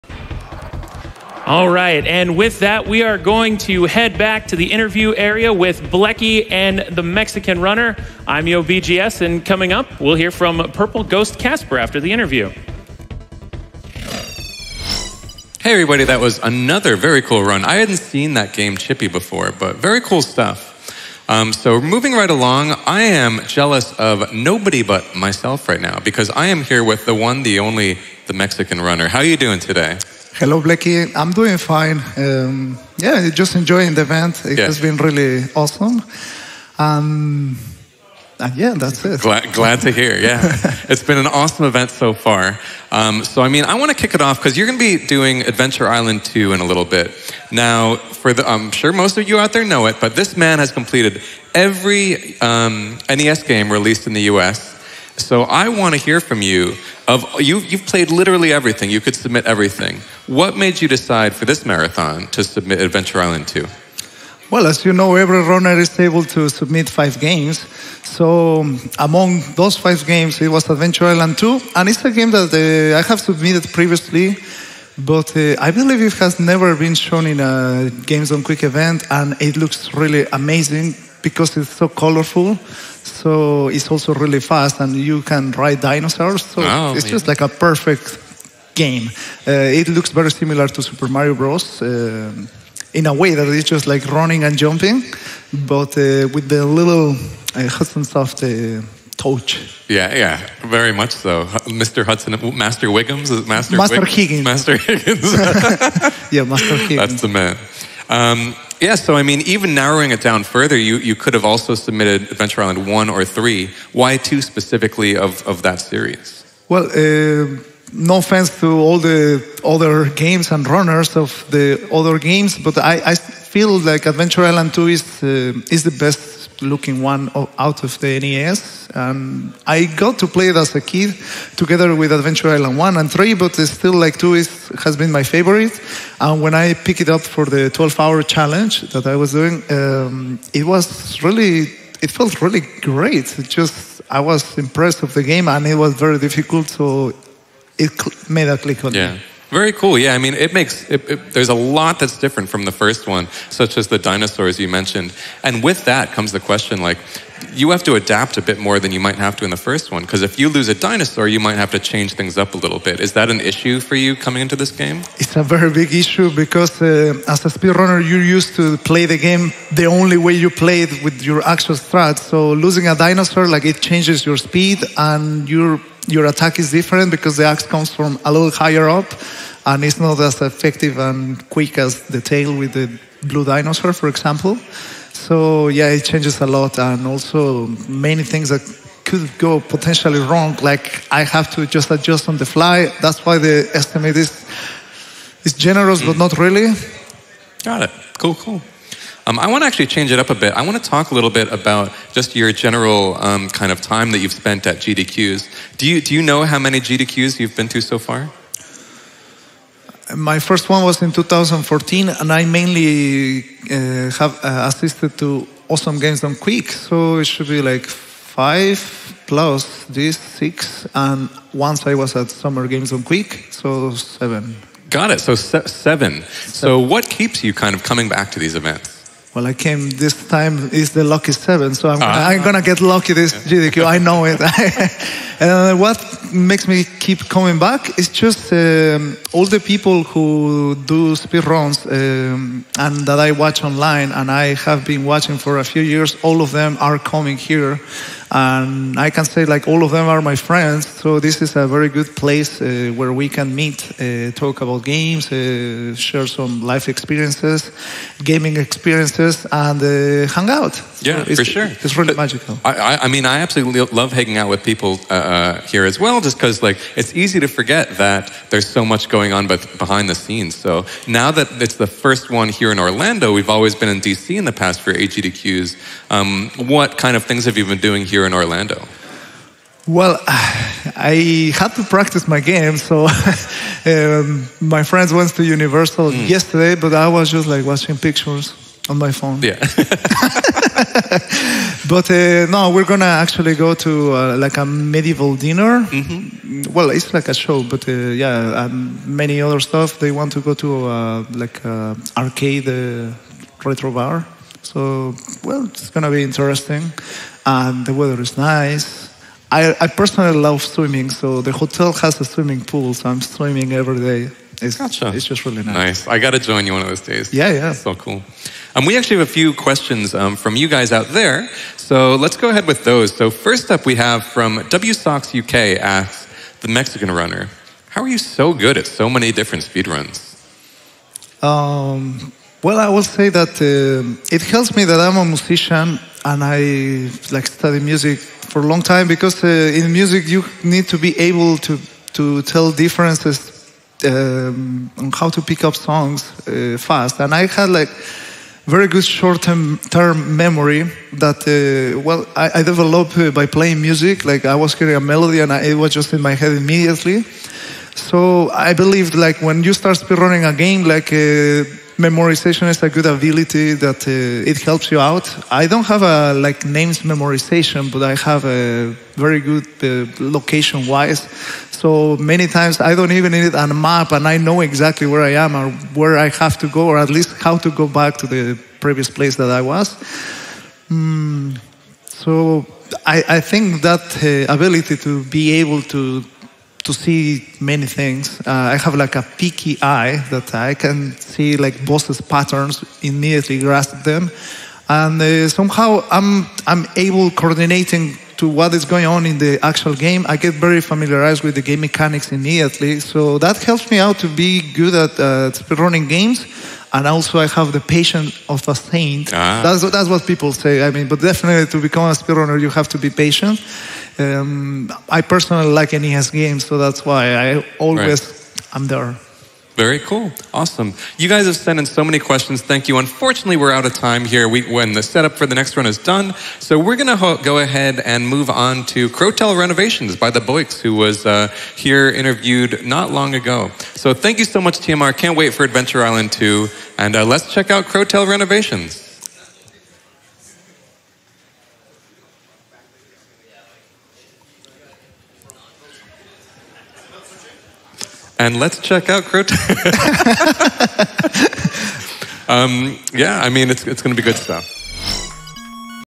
TheMexicanRunner interviewed by Blechy - AGDQ2020
This interview was recorded live in Orlando, Florida at AGDQ2020, a week long charity speedrun marathon raising money for the Prevent Cancer Foundation. Awesome Games Done Quick 2020 is just one of the many charity marathons put on by Games Done Quick. For more information on AGDQ2020, find them